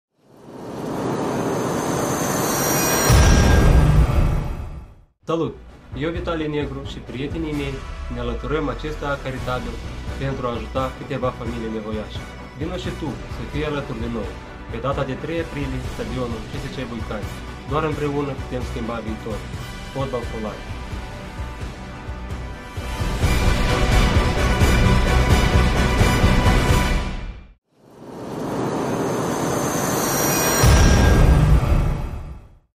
(CHARITY) Vitalie Negru before friendly game
Vitalie_NEGRU: Doar împreună putem schimba VIITORUL familiilor nevoiașe!

Pentru Olhionia, Vitalie Negru a evoluat 5 partide la Amoliga 2015, Liga 1, ediția de toamnă.

Video2 - Adresarea Puștiului Teribil, Ilie Ianachevici către elevii liceului Mircea Eliade din Chișinău

FCO este alături de cei mai triști ca noiI!
Puștii Teribili vor juca un meci amical cu scop CARITABIL!

Ce? Amical: Sine_Qua_Non
Prețul biletului: 25 de lei. Banii vor fi donați pentru 5 familii nevoiașe!

Unde? Chişinău, str. Ghidighici 1/2, CSCT Buiucani
Când? Duminică, 3 aprilie, ora 16.00.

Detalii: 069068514

Partener tehnic: Legea.md

Uniți pentru Victorie
Puștii Teribili